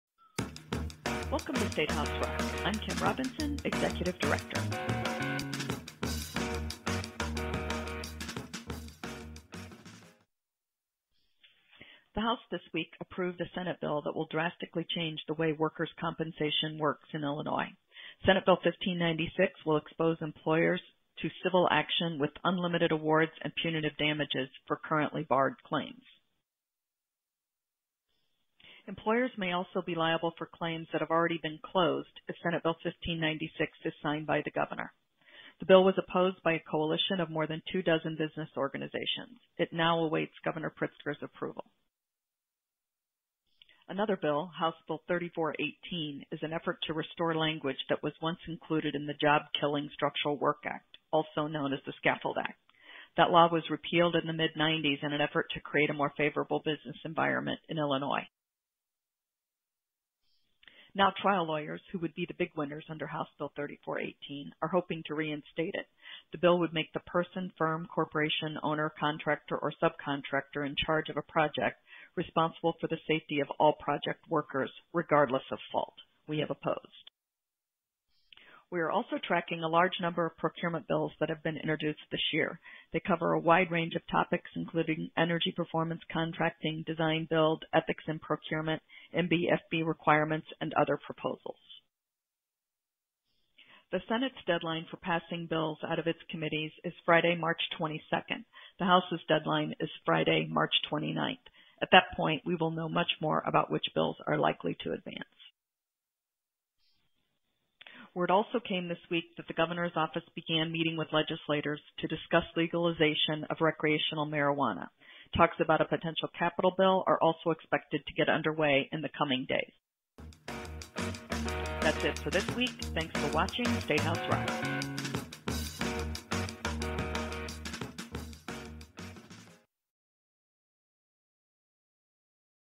shr 3.15.2019
In this episode, we discuss changes to the Illinois Worker's Compensation System, Revival of the Structural Work Act, Procurement as well as updates about legalization of recreational marijuana and  a capital bill.